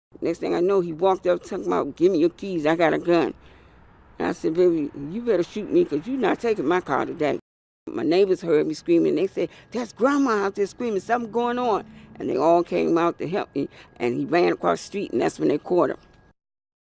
'Baby, you better shoot me': Teen ends up in ambulance after trying to carjack DC grandma
She was on her way to chemotherapy last week when a 15-year-old boy tried to carjack her, police said.

"Next thing I know, he walked up talking about, 'Give me your keys, I got a gun.' I said, 'Baby, you better shoot me, because you’re not taking my car," the woman, known as "Grandma" in her Southeast D.C. neighborhood, recounts to 7News reporter Sam Ford.

That's when the community stepped in.